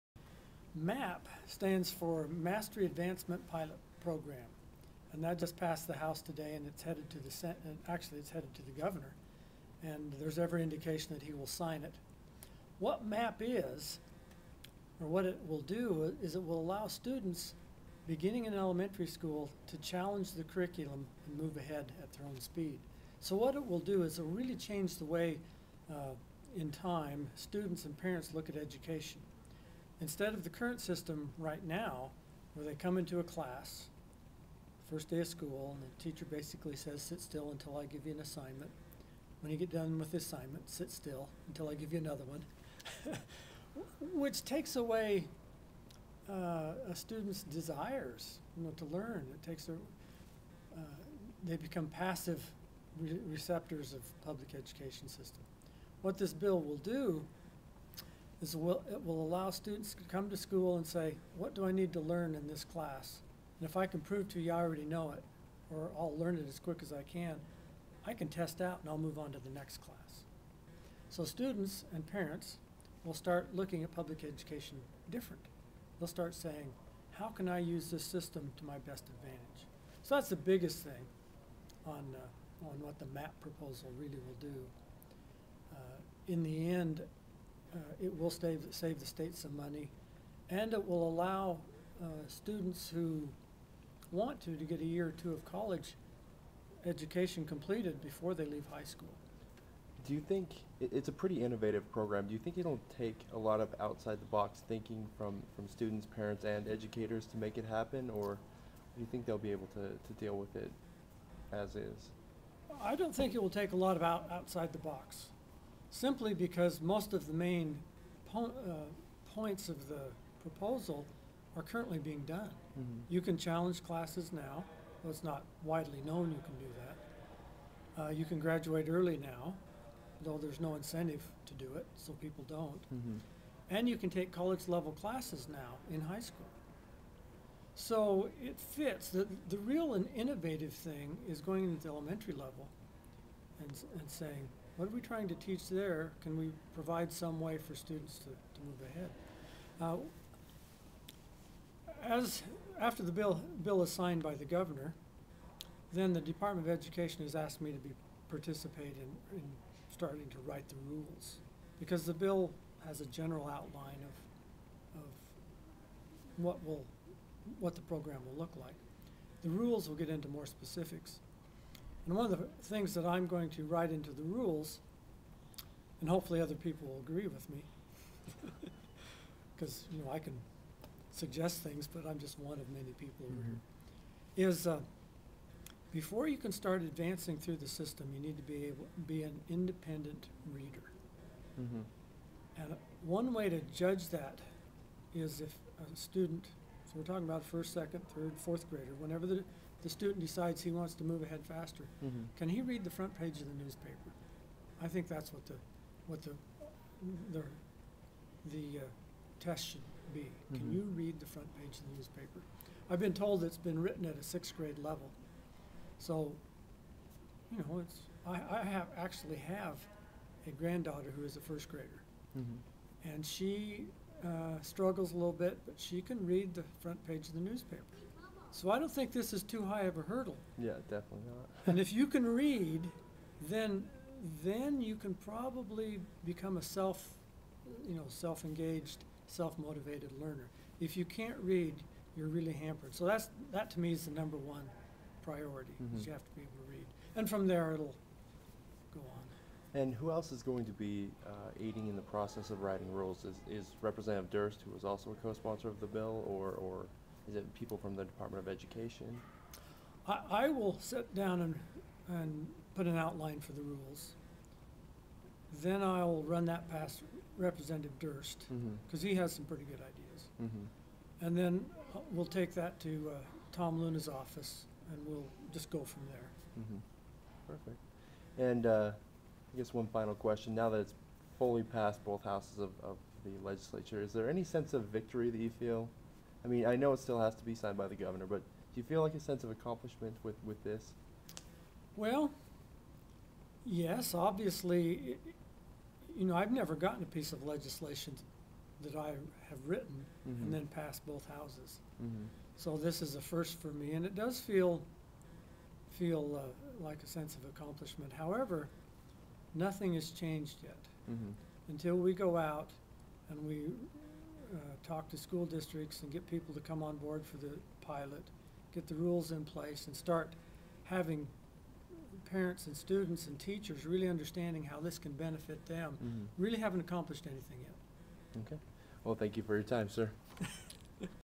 Rep. Steve Thayn talks about early graduation program
Rep. Steve Thayn of Idaho talks about his plan to allow students to graduate early from high school and receive a state-funded scholarship for doing so.